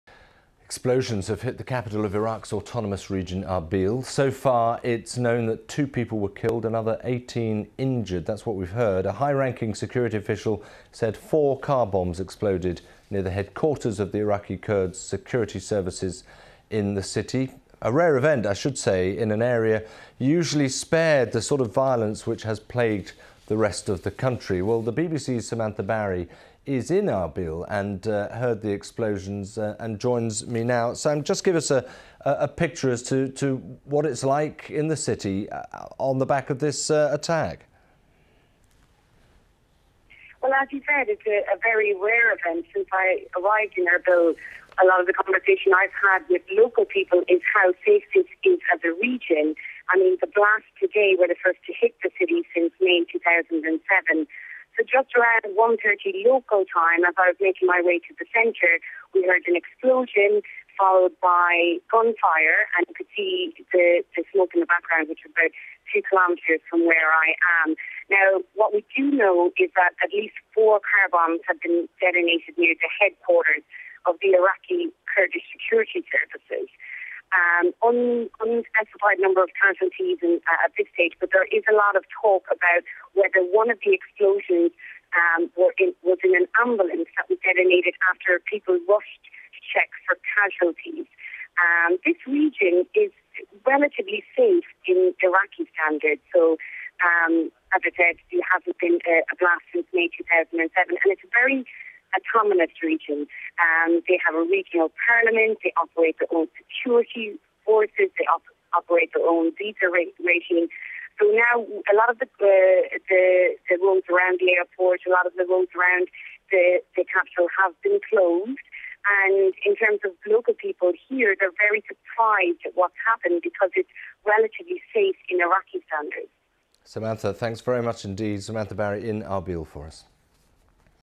CAR BOMBS IN IRBIL Samantha Barry reports from Iraq for BBC World News.
The Iraqi city of Irbil, the capital of the autonomous province of Kurdistan, has been rocked by a series of explosions killing two people and injuring 18.

Three car bombs and a roadside bomb targeted the security forces headquarters and the interior ministry.

Irbil is a stable part of Iraq and the region has not witnessed such attacks in the past six years.

Samantha Barry reports from Irbil.